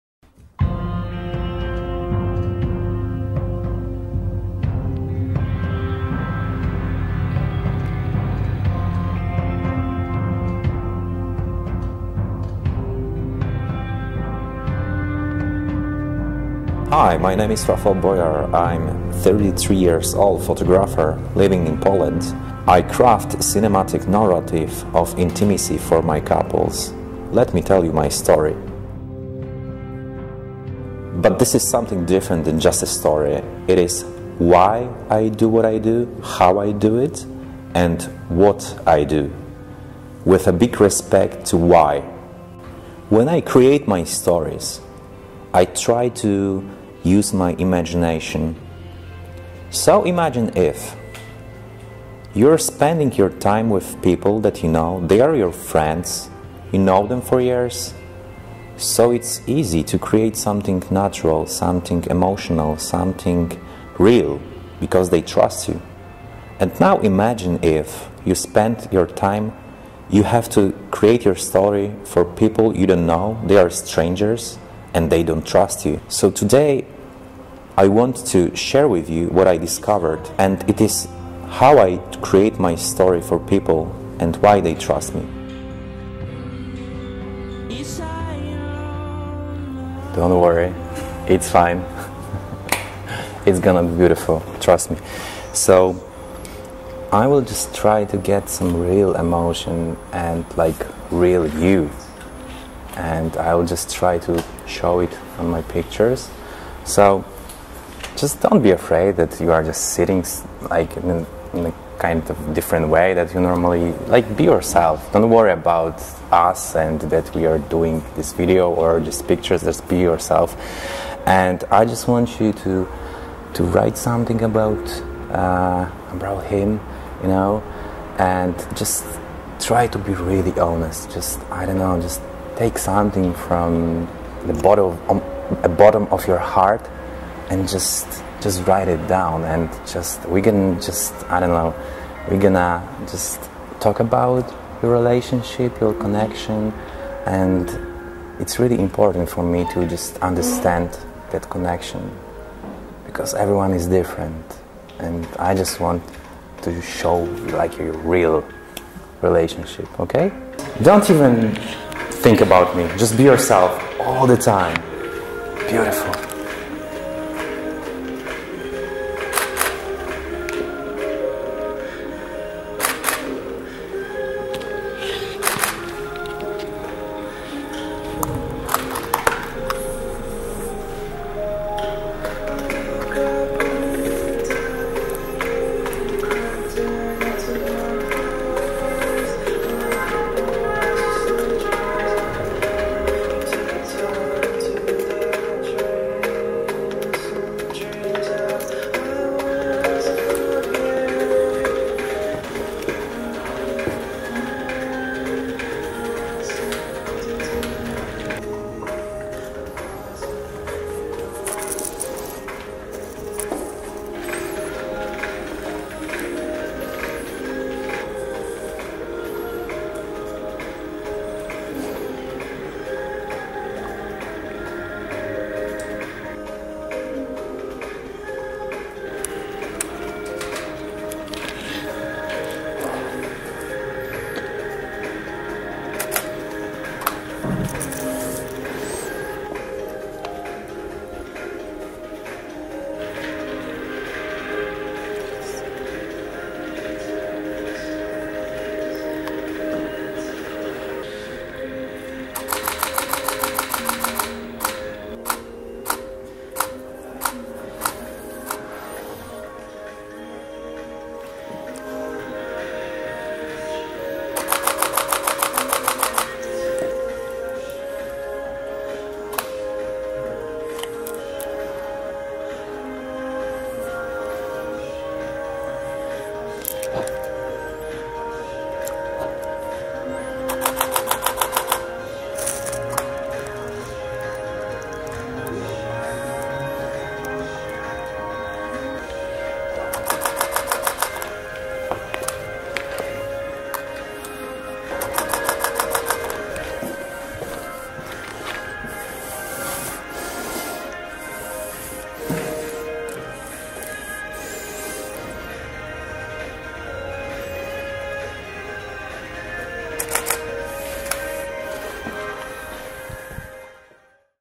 Wedding photography shooting with Rafal Bojar
Here's an introduction to Rafal Bojar's awesome shooting about creating cinematic intimate stories. Rafal has a unique vision for crafting intimate, beautiful stories. In order to set the mood that will allow these stories to unfold, he goes over a process with the couple that leads them to express their emotions and feelings in a deep way in front of the camera. Watch Rafal work with a real couple on an apartment, see how he creates the right atmosphere and learn about his approach on wedding & portrait photography and his philosophy on why what we do matters.